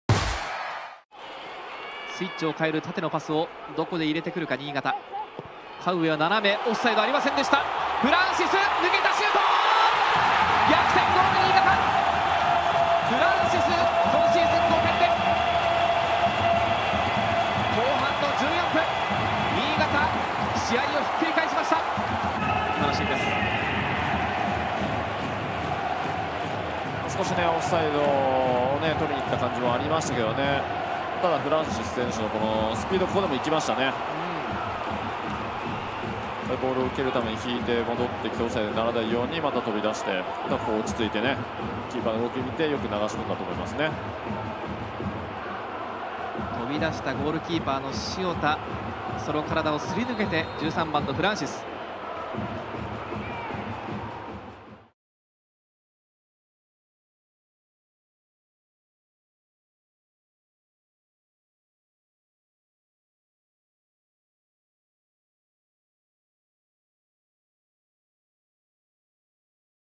【公式】ゴール動画：フランシス（新潟）59分 アルビレックス新潟vs大宮アルディージャ 明治安田生命Ｊ２リーグ 第21節 2019/7/6
2019年7月6日（土）に行われた明治安田生命Ｊ２リーグ 第21節 新潟vs大宮のフランシス（新潟）のゴール動画です。